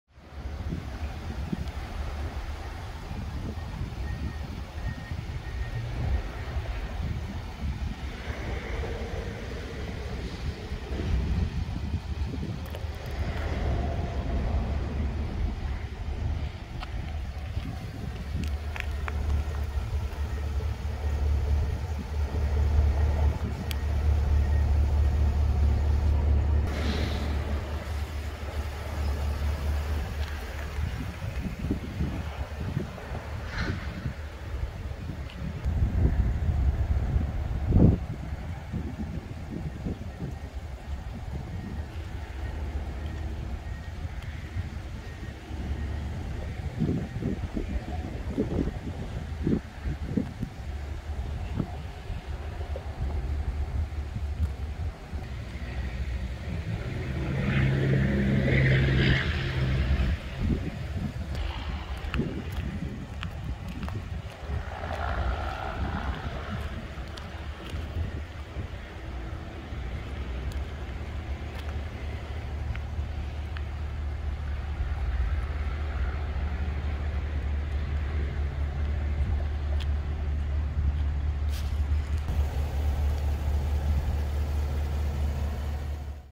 2023 Tahoe High Country - 22" SFO 6-Split Spoke Bright Chrome Rims
We upgraded the rims on this 2023 Tahoe High Country in Radiant Red Tintcoat to these awesome GM 22" SFO 6 Split Spoke Bright Chrome Rims and it looks so good! Part Number: 84799392